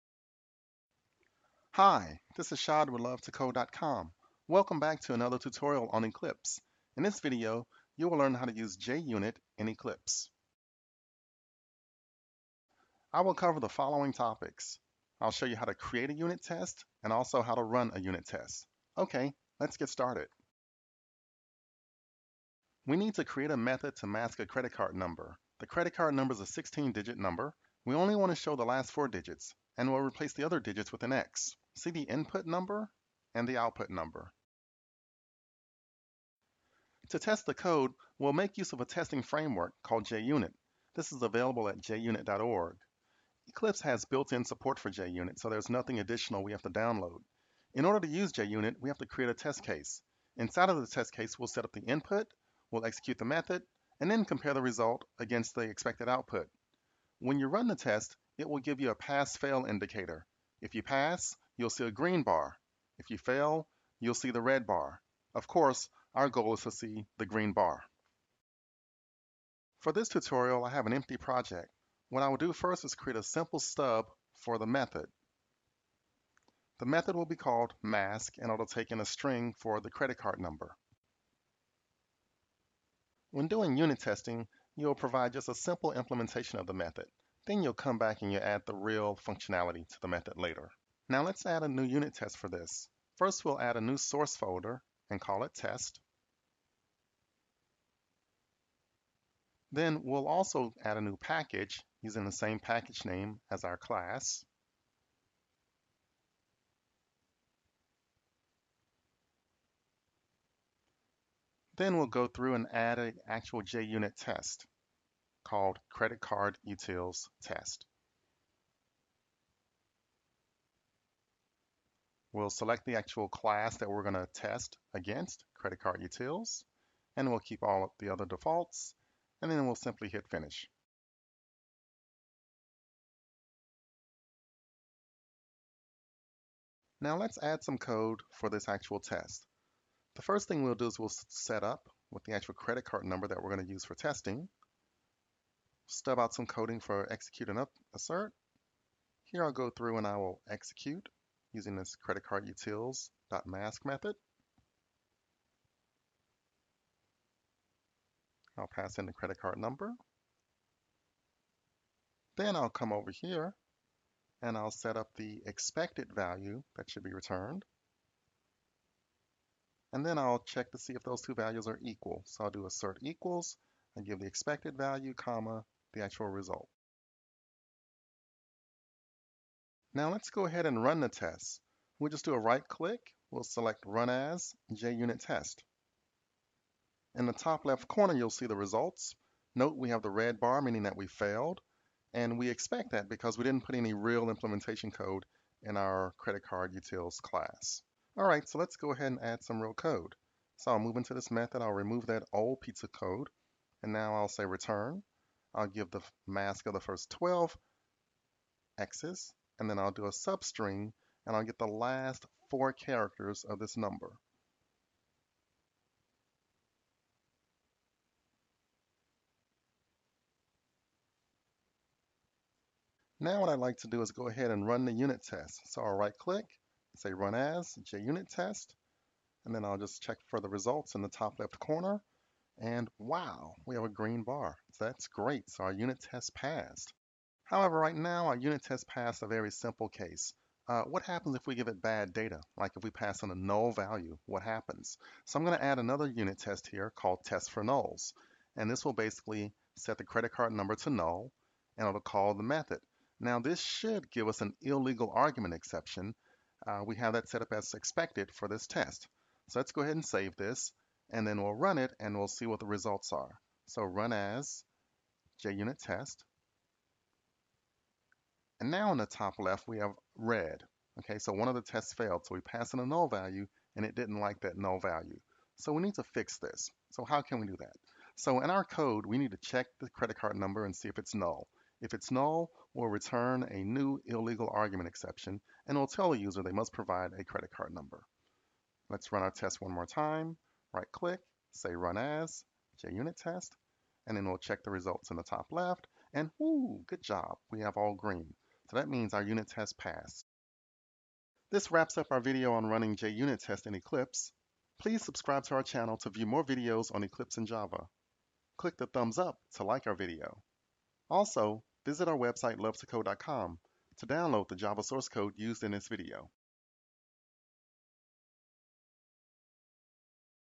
Java Eclipse Tutorial - Part 9: Running JUnit Tests
NEED TO LEARN JAVA - 5 FREE JAVA VIDEO COURSES

In this video tutorial, I will show you how to run JUnit tests Eclipse. I will discuss the following topics:
• Create a Unit Test
• Running the Test

Video Transcript
Time - 00:00
Hi, this is Chad (shod) with luv2code.com. Welcome back to another tutorial on Eclipse. In this video, you'll learn how to use JUnit in Eclipse.

Time - 00:12
I'll cover the following topics: I'll show you how to create a unit test and also how to run a unit test. Okay. Let's get started.

Time - 00:23
We need to create a method to mask a credit card number. The credit card number is a 16-digit number. We only want to show the last 4 digits, and we'll replace the other digits with an X. See the input number and the output number.

Time - 00:35
To test the code, we'll make use of a testing framework called JUnit. This is available at junit.org. Eclipse has built-in support for JUnit, so there is nothing additional we have to download. In order to use JUnit, we have to create a test case. Inside of the test case, we'll setup the input, we'll execute the method, and then compare the result against the expected output. When you run the test, it will give you a pass-fail indicator. If you pass, you'll see the green bar. If you fail, you'll see the red bar. Of course, our goal is to see the green bar.

Time - 01:09
For this tutorial, I have an empty project. What I'll do first is create a simple step for the method. The method will be called Mask and it will take a string for the credit card number. When doing unit testing, you'll provide just a simple implementation of the method. Then, you'll come back and you'll add the real functionality to the method later.

Time - 01:31
Now, let's add a new unit test for this. First, we'll add a new source folder and call it test. Then, we'll also add a new package using the same package name as our class. Then, we'll go through and add an actual JUnit test called CreditCardUtilsTest. We'll select the actual class that we're going to test against CreditCardUtils, and we'll keep all of the other defaults and then we'll simply hit Finish.

Time - 02:16
Now, let's add some code for this actual test. The first thing we'll do is we'll setup with the actual credit card number that we're going to use for testing. Stub out some coding for executing assert. Here, I'll go through and I'll execute using this credit CreditCardUtils.mask method.

Time - 02:38
I'll pass in the credit card number. Then, I'll come over here and I'll setup the expected value that should be returned, and then I'll check to see if those two values are equal, so I'll do assertEquals and give the expected value, the actual result.

Time - 02:59
Now, let's go ahead and run the test. We'll just do a right click. We'll select Run As and JUnit test. In the top left corner, you'll see the results. No, we have the red bar meaning that we failed, and we expect that because we didn't put any real implementation code in our CreditCardUtils class.

Time - 03:18
All right, so let's go ahead and add some real code. I'll move into this method. I'll remove that old piece of code and now I'll say return. I'll give the mask of the first 12 x's and then I'll do a substring, and I'll get the last 4 characters of this number.

Time - 03:43
Now, what I like to do is go ahead and run the unit test. I'll right click, say Run As, JUnit test, and then I'll just check for the results in the top left corner, and wow we have a green bar. That's great, so our unit test passed.

Time - 03:58
However, right now our unit test passed a very simple case. What happens if we give it bad data? Like we pass on the no value. What happens? I'm going to add another unit test here called test_nulls, and this will basically set the credit card number to null and it'll call the method.

Time - 04:12
Now, this should give us an IllegalArgumentException. We have that setup as expected for this test, so let' go ahead and save this and then we'll run it and we'll see what the results are. Run As, JUnit test, and now on the top left we have red, okay. One of the tests failed, so we passed in a null value and it didn't like that no value. We need to fix this, so how can we do that?